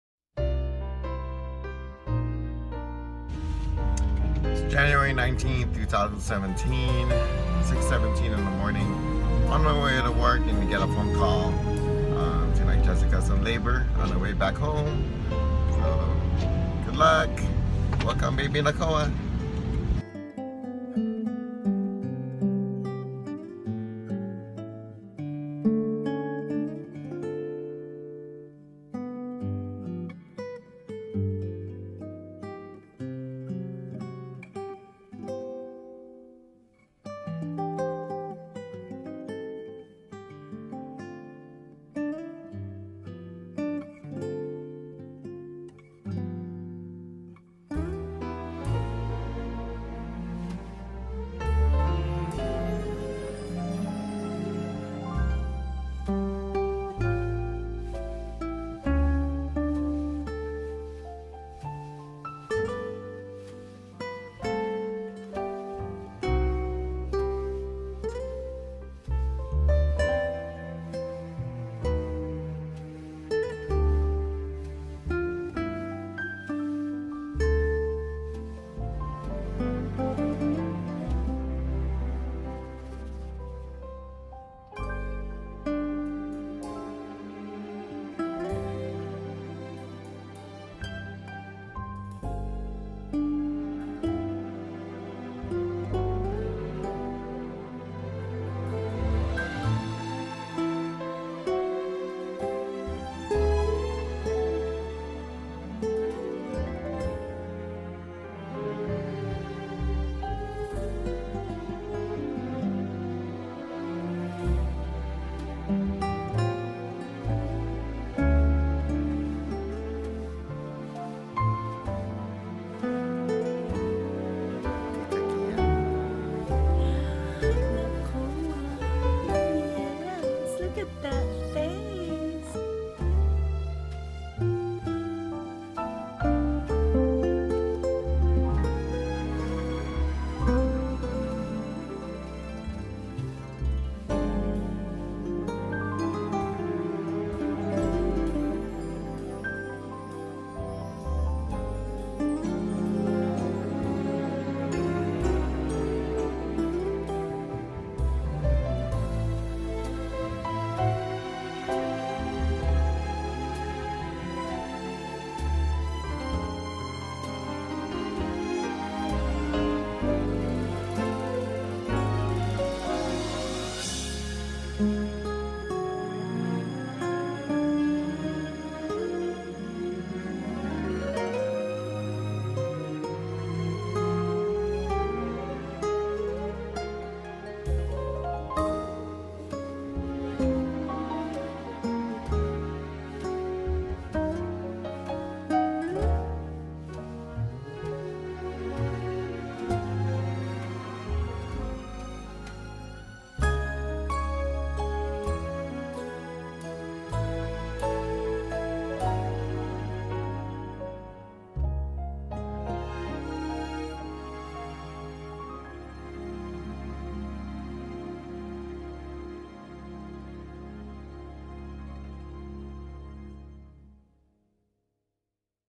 Nakoa Michael Joseph Tedesco
Born: January 19, 2017 1:48pm 9lbs 20inches

Nakoa (Hawaiian name meaning "Brave")
Michael (in honor of our good friend Michael Sharpe who passed away 5 years ago today from leukemia)

We would like to thank everyone for all the best wishes via texts, FB, and IG comments.
This video was compiled from my SnapChats, videos and pics.